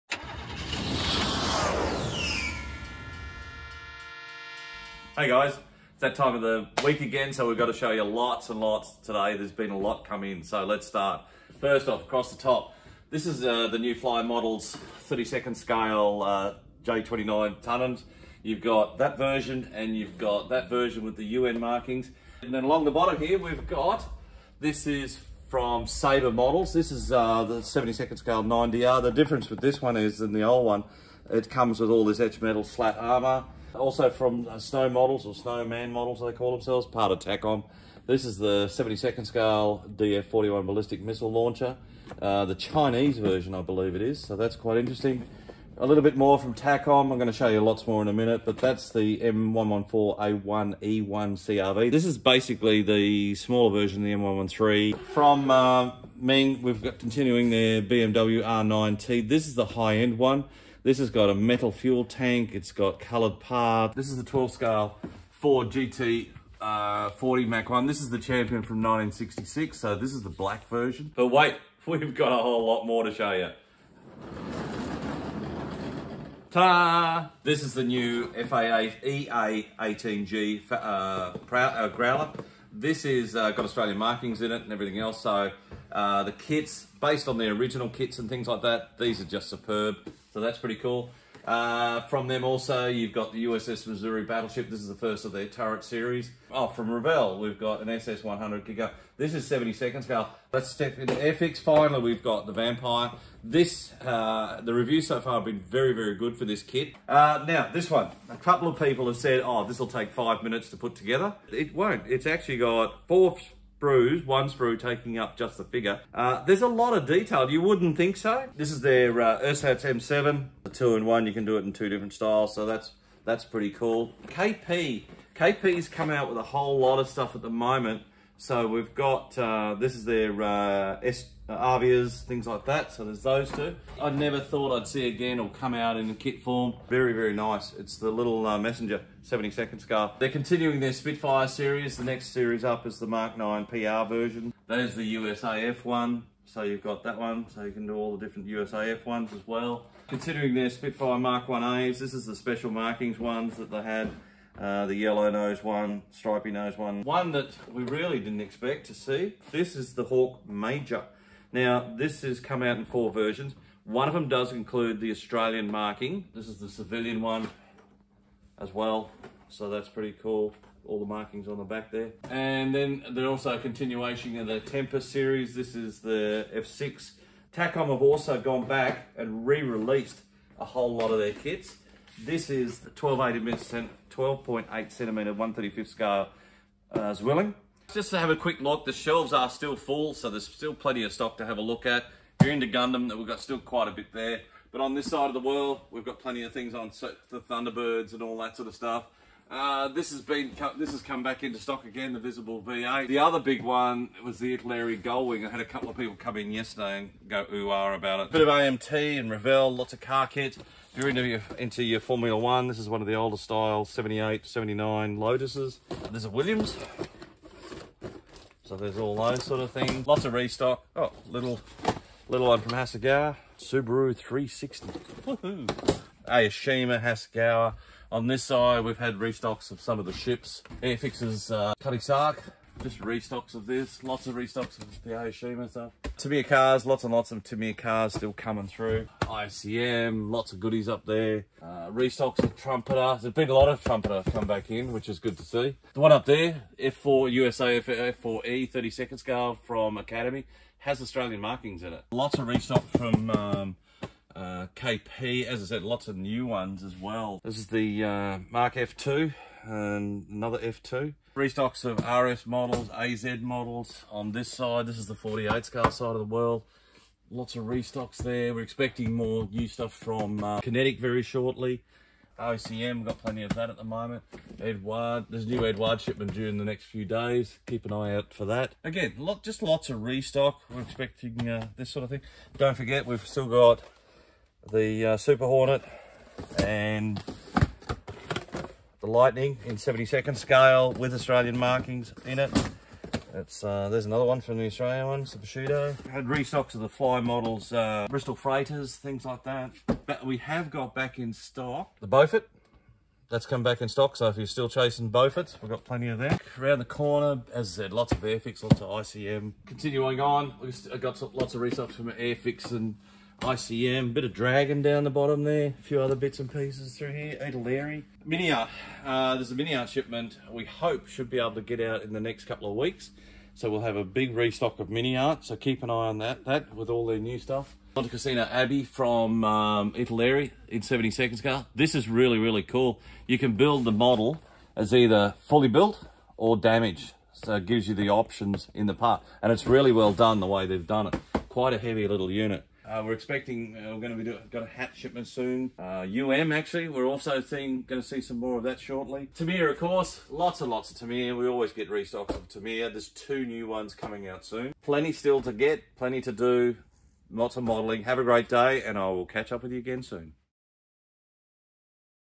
All the New Model Kits on the Shelf for the Weekend | Model Kit News
Kits, kits, and even more kits! Max is going left and right to point you to the right direction for what is a cool kit project, or the next addition to your kit collection! So much good stuff on the shelf, we are pulling all the stops to show you EVERYTHING! Hang on tight and here we go!